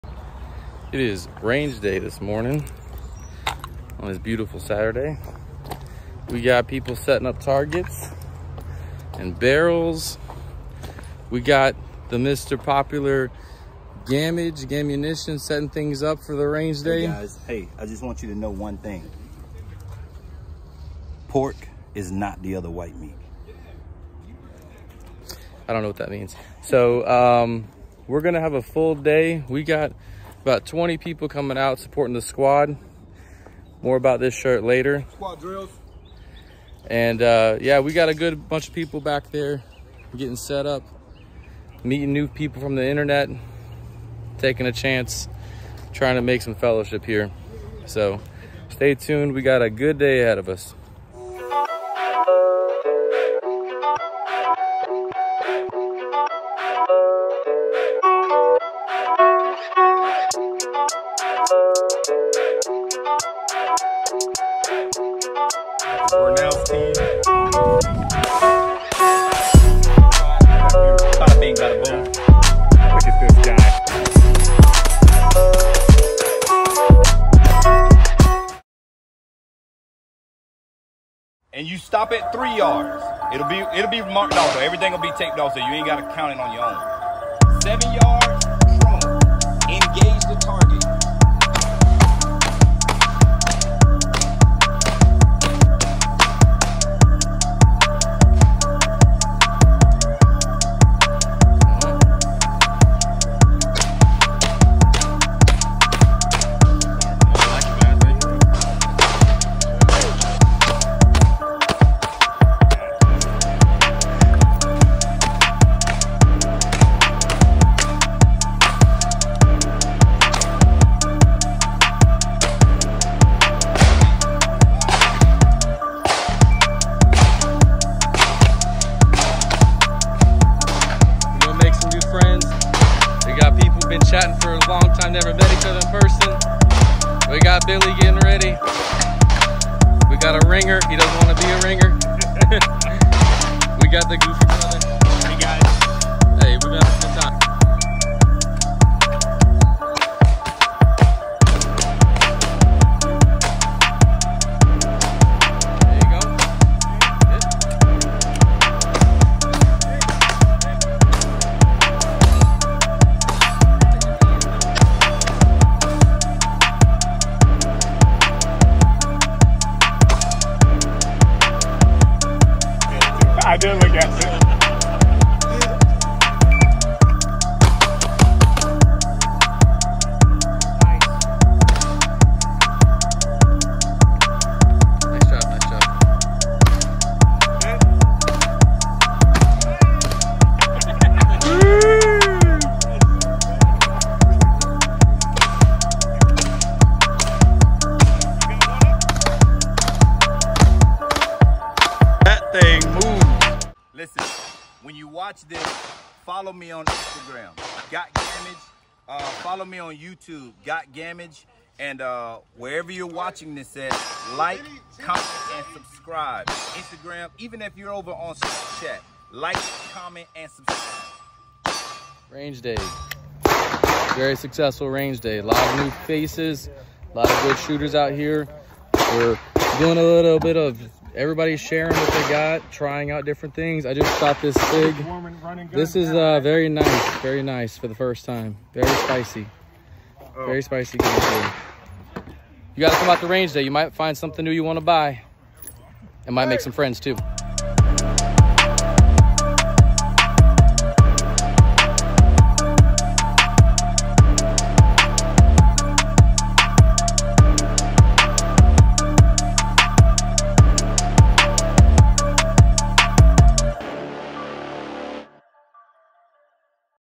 Range Day April 2022
Hi There! Thanks for checking out my video.

Today we had our first Squad Drills Range Day. We had over 20 people out moving and shooting and making memories and new friendships. This is a recap of some of the drills we ran.

Checkout my squad at the links below for some more fun content.